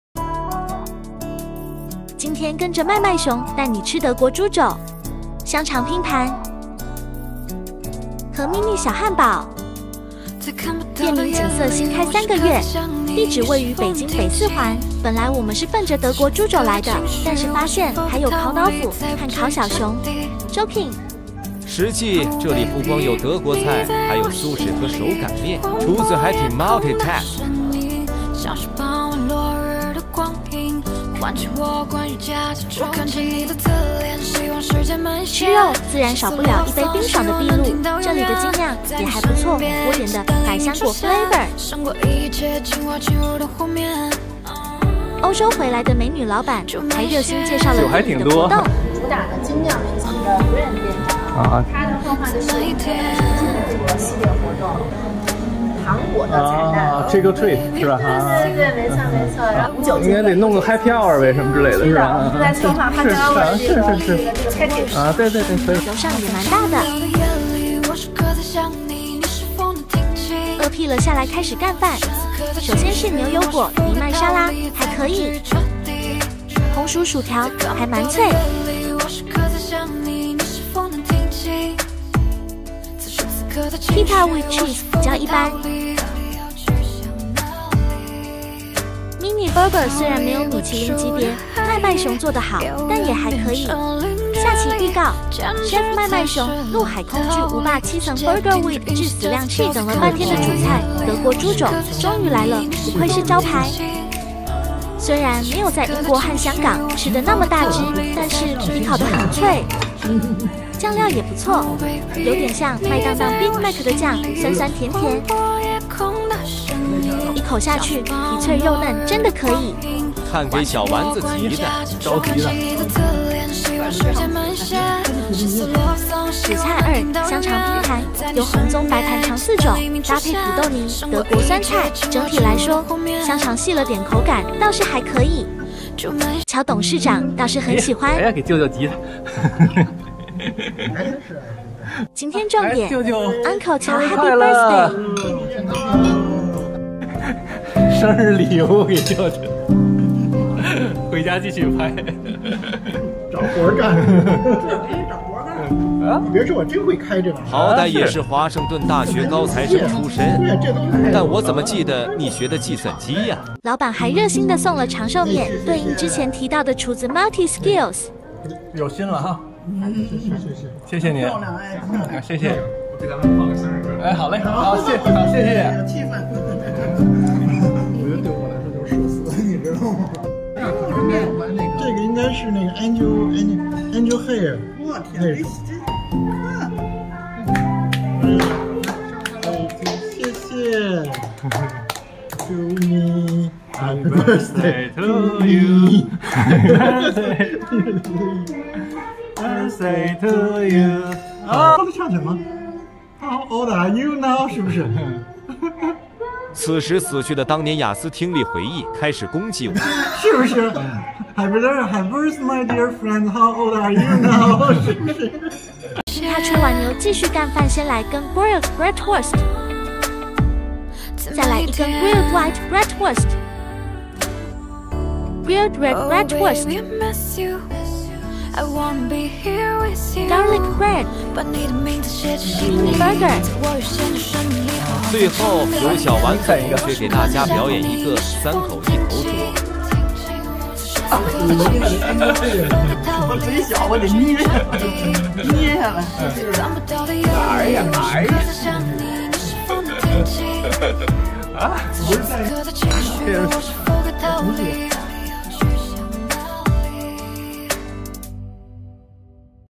Happy Birthday Uncle Qiao"Sushi Samba" Schweinshaxe .German roast pork knuckle & Sausage platter
"景啬" 德国猪肘 香肠拼盘 Uncle 乔 生日快乐
Happy Birthday Uncle Qiao
"Sushi Samba" schweinshaxe .
German roast pork knuckle & sausage platter